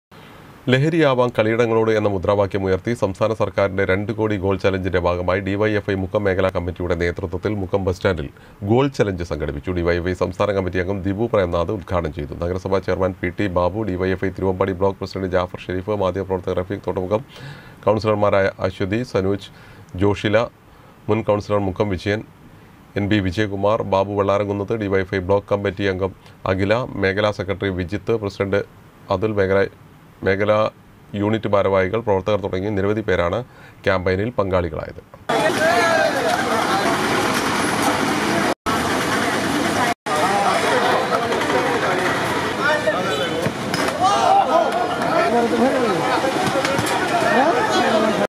സംസ്ഥാന സർക്കാരിന്റെ 2 കോടി ഗോൾ ചലഞ്ചിന്റെ ഭാഗമായി DYFI
ലഹരിയാവാം കളിയിടങ്ങളോട് എന്ന മുദ്രാവാക്യം ഉയർത്തി സംസ്ഥാന സർക്കാരിന്റെ 2 കോടി ഗോൾ ചലഞ്ചിന്റെ ഭാഗമായി DYFI മുക്കം മേഖലാ കമ്മിറ്റിയുടെ നേതൃത്വത്തിൽ മുക്കം ബസ് സ്റ്റാൻഡിൽ ഗോൾ ചലഞ്ച് സംഘടിപ്പിച്ചു